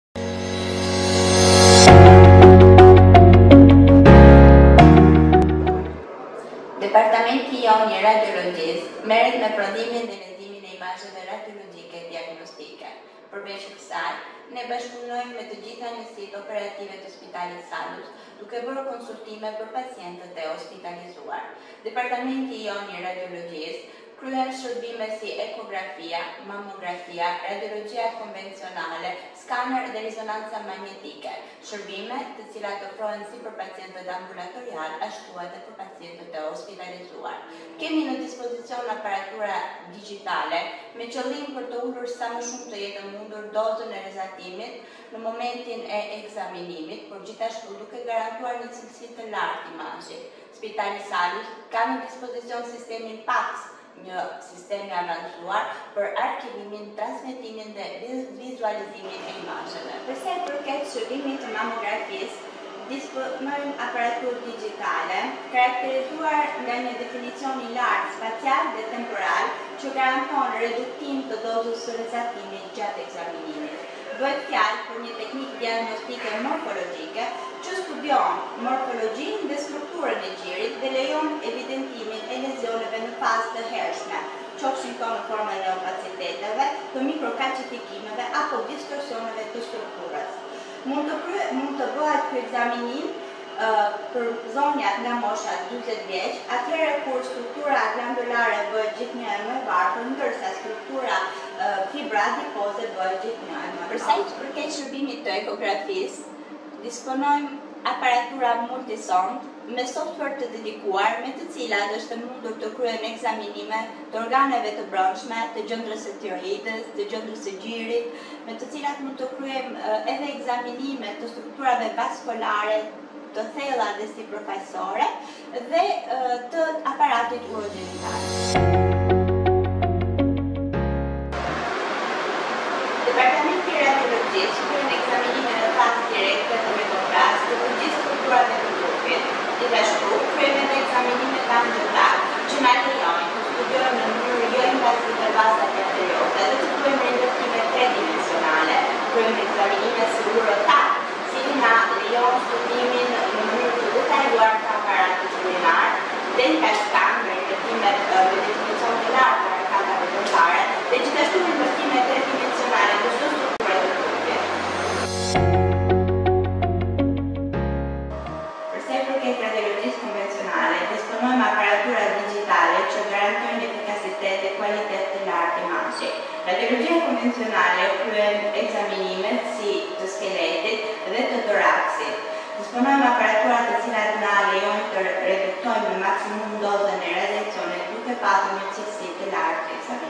Departamenti i Radiologjisë në Salus
Departamenti jone i Radiologjisë merret me prodhimin dhe leximin e imazheve radiologjike diagnostike.
Përveç kësaj, departamenti bashkëpunon me të gjitha Njësitë Operative të Spitalit SALUS, duke bërë konsultime për pacientët e hospitalizuar.
Departamenti jonë i Radiologjisë garanton shërbime si ekografia, mamografia, radiologjia konvencionale, skaner dhe rezonancë magnetike, shërbime të cilat ofrohen për pacientet në regjim ambulatorial dhe pacientë të hospitalizuar.

Kemi në dispozicion aparatura digitale me qëllim për të ulur sa më shumë të jetë e mundur ekspozimin ndaj rrezatimit gjatë ekzaminimit duke garantuar një kualitet të lartë imazhi.

Spitali Salus ka në dispozicion sistemin PACS, një sistem për arkivimin, transmetimin dhe vizualizimin e imazheve diagnostike digitale.

ECOGRAFIA
Disponojmë apartura multisonde, me software të dedikuar, me të cilat ekzekutohen ekografi të organeve të brendshme, të gjëndrës tiroide, të gjirit, të traktit urogjenital, të strukturave vaskolare sipërfaqesore dhe të thella.

Mammografia
Disponojmë aparatura digitale të karakterizuara nga një definicion i lartë spacial dhe temporal, që garanton reduktim të rrezatimit gjatë ekzaminimit.
Bëhet fjalë për një teknikë diagnostike morfologjike, që studion formën dhe strukturën e gjirit dhe lejon evidentimin e lezioneve në fazë të hershme qofshin këto në formën e opaciteteve, mikrokalcifikime apo distorsionet e strukturës.
Mund të niset kryerja e ketij ekzaminimi që në moshen 40 vjeç, kohë në të cilën gjëndra mamare bëhet gjthnjë e më e pasur me komponent dhjamor dhe më e varfër nga komponenti glandular.

Radiologia Konvenzionale
Disponojmë aparatura digitale që garantojnë efikasitet të lartë  dhe kualitet të lartë imazhi.
Radiologjia konvencionale kryen ekzaminime të skeletit dhe të toraksit me aparatura që na lejojnë të reduktojmë dozën e rrezatimit duke mbajtur një kualitet të lartë imazhi.

TC
Departamenti i radiologjisë kryen ekzaminime në fazë direkte dhe me kontrast të të gjithë strukturave të trupit. Gjithashtu kryejmë ekzaminime Angio-Tc që na lejojnë të studiojmë në mënyrë jo invazive vazat arterioze edhe me rindërtime 3 dimensionale, URO-TC që na lejon të studiojmë në mënyrë të detajuar aparatin urinar, Dentalscan që rindërtojnë harkatat dentare me një definicion të lartë, dhe rindërtime 3 dimensionale të çdo strukture të trupit.

REZONANCA MAGNETIKE
Struktura jonë disponon aparaturë 1.5 Tesla, që na lejon të kryejme ekzaminime direkte dhe me kontrast të sistemit nervor (si truri, hipofiza, orbitata dhe kolona vertebrale), të qafës, abdomenit, pelvisit, përfshirë këtu dhe colangio-RM per studimin e rrugëve biliare, ekzaminime osteo-artikolare dhe muskolo-tendinoze të të gjithë distrekteve.